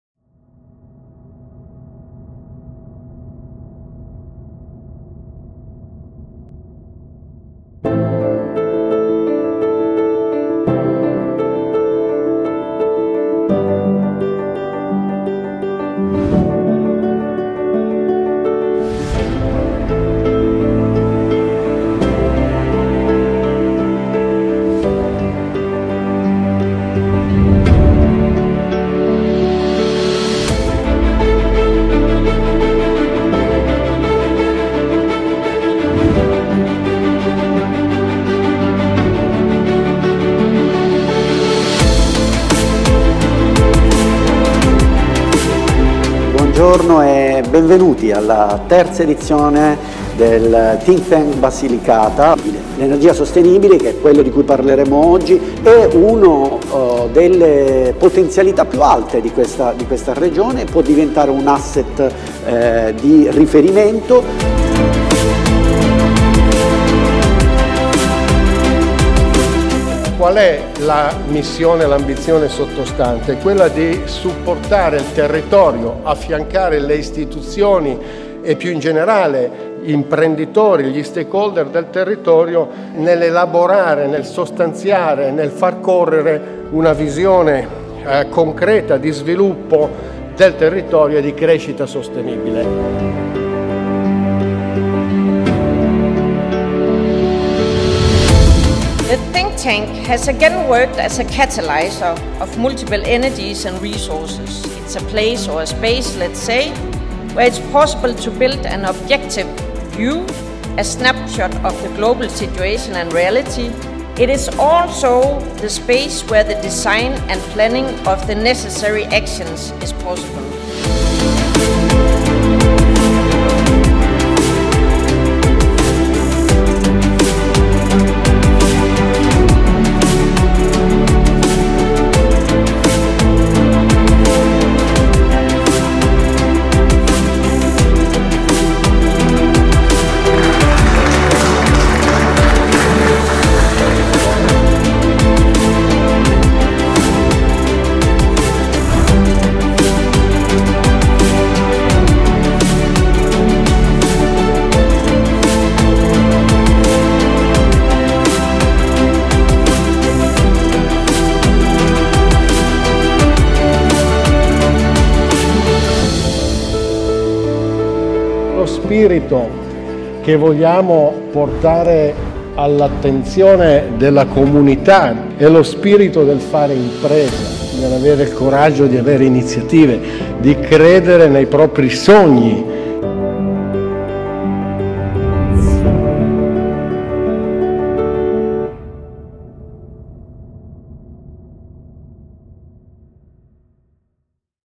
Forum Think Tank Basilicata 2022
Forum “Energie per un futuro sostenibile”. Matera, 25 febbraio 2022
Il Think Tank Basilicata è una iniziativa che TotalEnergies EP Italia ha fondato insieme ai partners della Joint venture Tempa Rossa Shell e Mitsui e con il contributo scientifico di The European House – Ambrosetti nel 2018